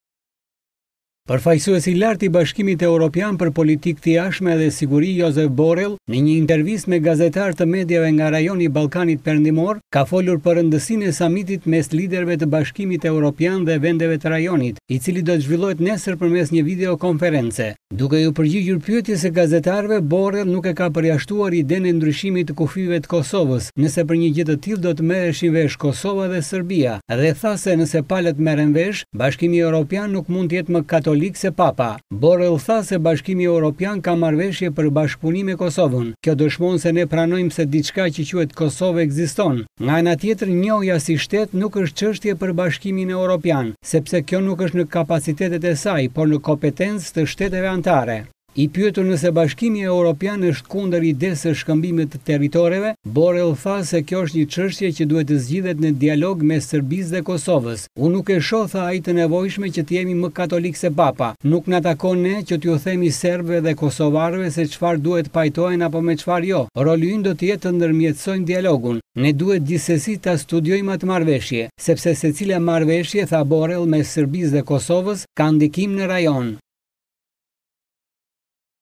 Borrell nuk është kundër ndryshimit të kufijve të Kosovës
Në një intervistë me katër gazetarë të mediave nga rajoni i Ballkanit Perëndimor, përfshirë edhe grupin KOHA, Përfaqësuesi i lartë i BE-së për Politikë të Jashtme dhe Siguri, Josep Borrell, ka folur për rëndësinë e samitit mes liderëve të BE-së dhe vendeve të rajonit që zhvillohet nesër përmes video-lidhjes. Ai, duke u përgjigjur në pyetje të gazetarëve, nuk e ka përjashtuar as idenë e ndryshimit të kufijve të Kosovës nëse për një gjë të tillë do të merreshin vesh Kosova dhe Serbia. Ai ka thënë se. nëse palët merren vesh, “BE nuk mund të jetë me katolik sesa Papa”.